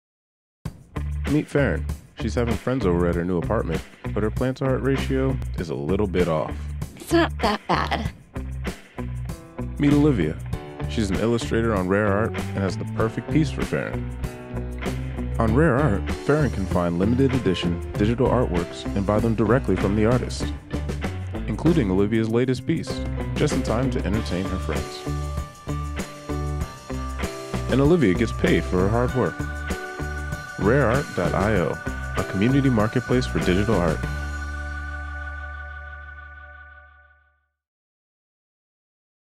RAREart.io: Discover, Collect & Display Limited-Edition Digital Art (Captioned)
RAREart.io unlocks the way the way for people to discover, collect, and display authentic & limited-edition digital art, verifiable on the blockchain. To help you understand our model, we’ve created this video to create a more vivid image for you on how digital art transactions operate.

If you're ready to start shopping, make sure to hit ASAP.